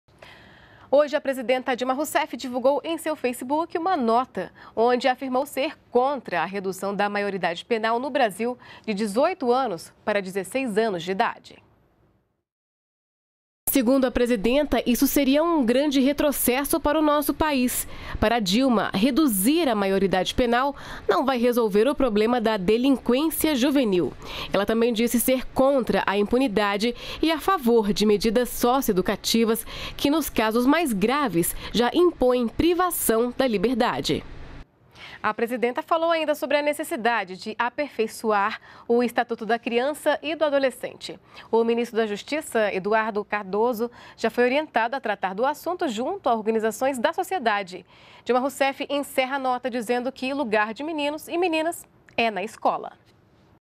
Dilma Rousseff defende posição contrária à redução da maioridade penal no país
NBR NOTÍCIAS - 13.04.15: Nesta segunda-feira (13), a presidenta Dilma Rousseff manifestou, em seu perfil oficial do Facebook, sua posição contrária à redução da maioridade penal que está em discussão no Congresso Nacional. A proposta pretende reduzir de 18 para 16 de anos de idade a maioridade penal no Brasil. Segundo a presidenta, a medida seria um retrocesso e não resolveria o problema da delinquência juvenil. Dilma disse também ser contra a impunidade e a favor de medidas socioeducativas.